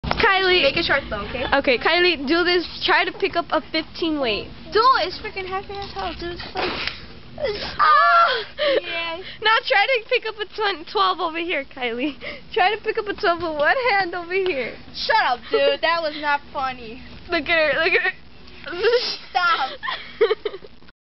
Im weak !
omq ! serina made me pick up like a 100 lb weiqhtt !
it waz heavy az hell ! uqh ! but funny:]